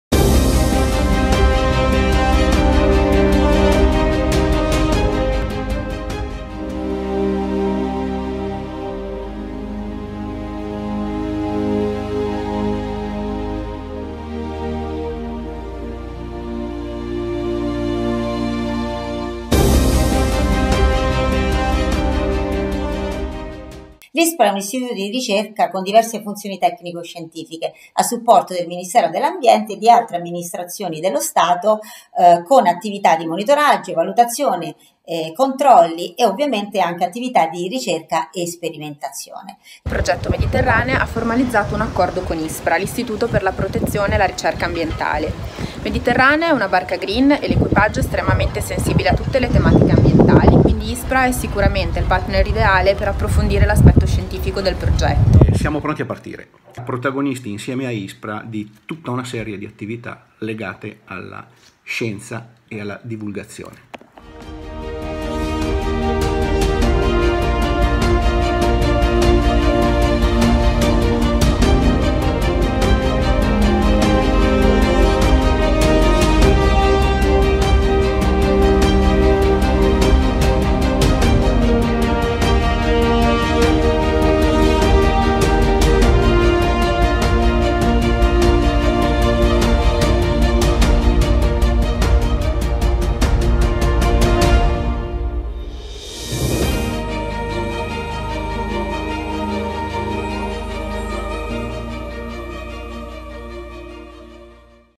Progetto Mediterranea e ISPRA insieme per il mare
Accordo di collaborazione tra l'Istituto Superiore per la Protezione e la Ricerca Ambientale (ISPRA) e l'Associazione Progetto Mediterranea per sviluppare un'attività di citizen science al fine di raccogliere informazioni ambientali a supporto dei programmi di monitoraggio ambientale con un processo partecipativo.